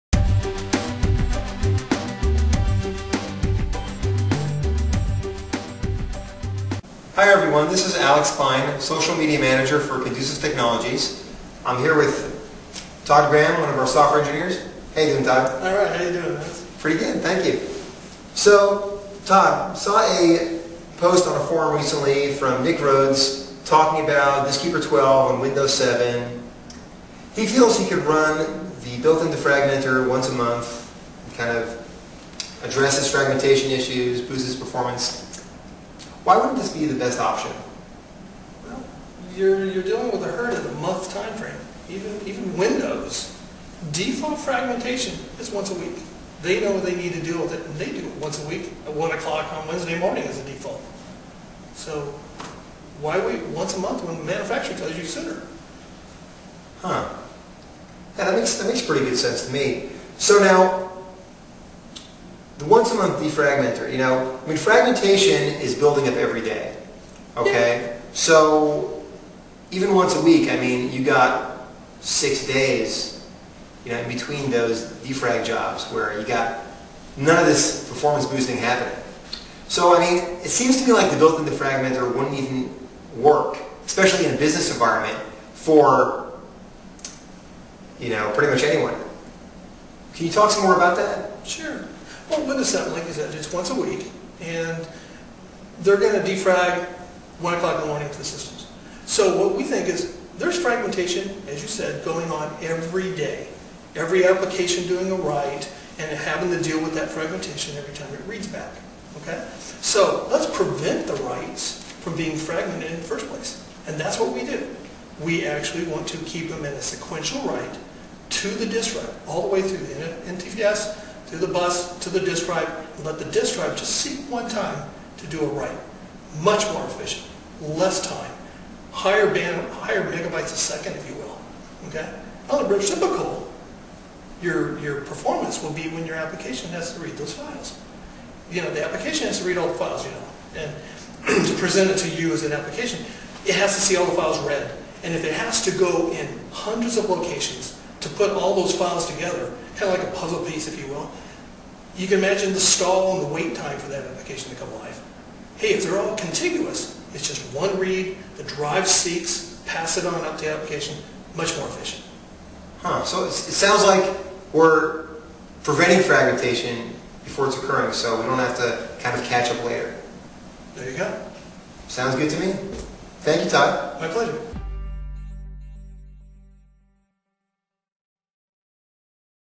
Forum Post Response: Windows 7 and Diskeeper 12
This is a response to the following post on WildersSecurity.com by "Nick Rhodes":

Quote:

Originally Posted by AlexKlein
The bottom line is this: who wants to spend time with manual defrag jobs? Why would you want to monitor your fragmentation levels and defrag manually when you see necessary?

Think of your car. What if your car monitored your oil levels on it's own and added or "changed" the oil for you on an ongoing basis - consistently keeping your car running at top performance - without tying down resources?

Alex
Condusiv Technologies (Formerly Diskeeper Corp)

Nick's response:

If the oil offers sufficient protection between services, there is no benefit to changing the oil on an ongoing basis.

Why pay extra to install this system (akin to Diskeeper) when you get a lifetime of free servicing (akin to built in defrag of Windows) ?

Cheers, Nick.